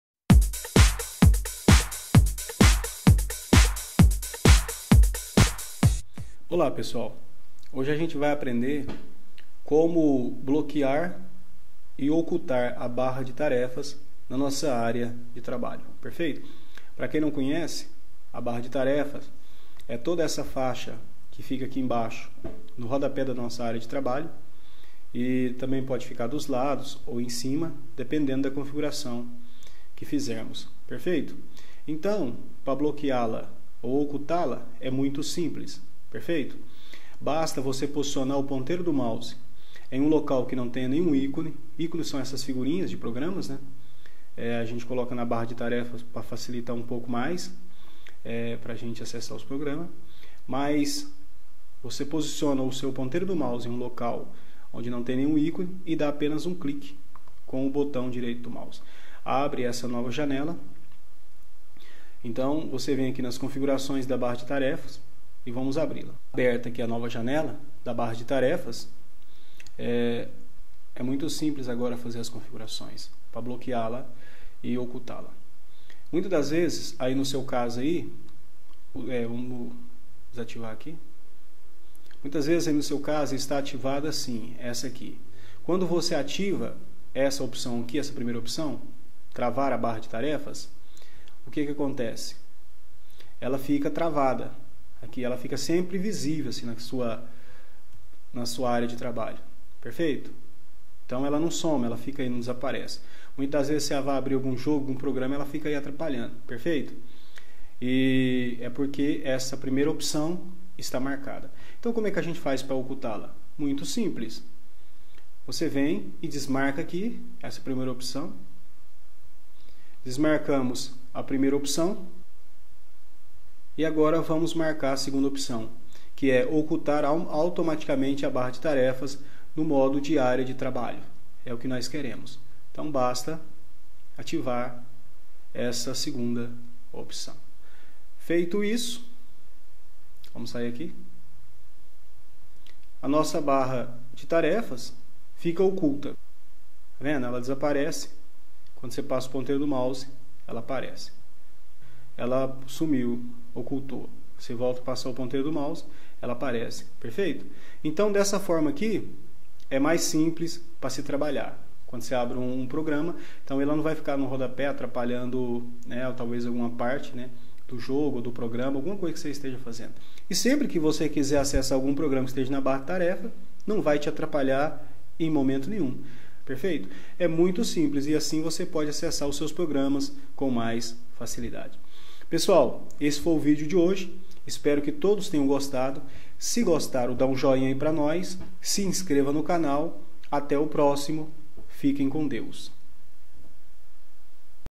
Como ocultar e bloquear a barra de tarefas no Windows 10 para criadores
A configuração correta do sistema Windows é o segredo para o alto desempenho do computador.